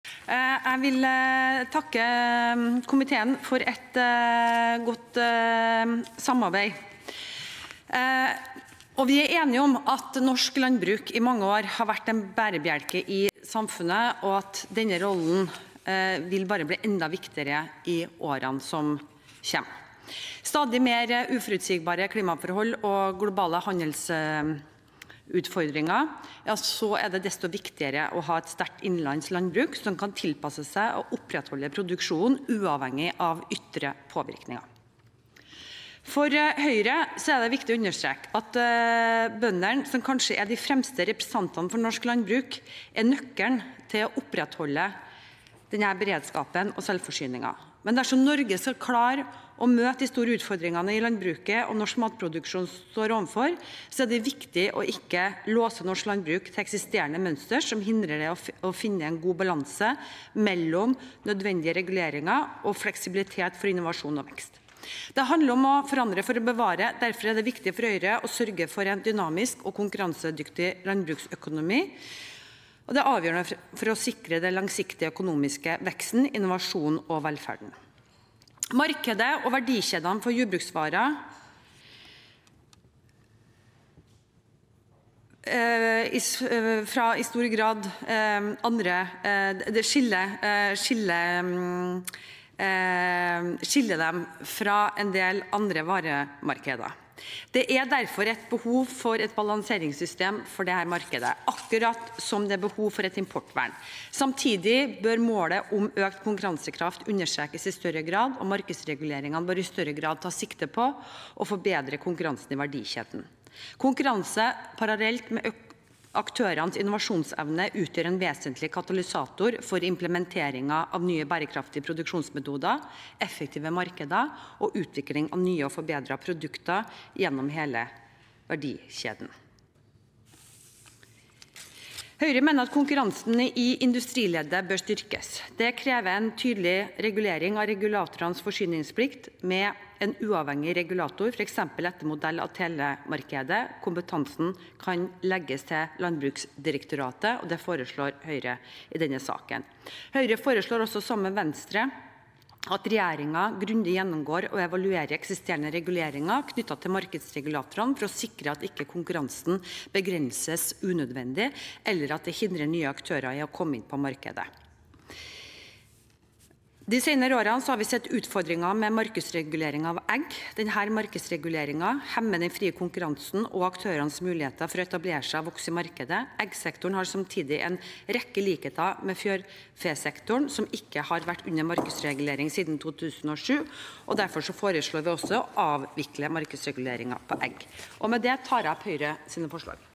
29.04.2024 Sak 5 1 Linda Hofstad Helleland
Om endringer i markedsregulatorordningene i landbruket for å bidra til et rettferdig system som både sikrer produksjonsgrunnlag og sunn konkurranse (Innst. 262 S (2023–2024), jf. Dokument 8:91 S (2023–2024))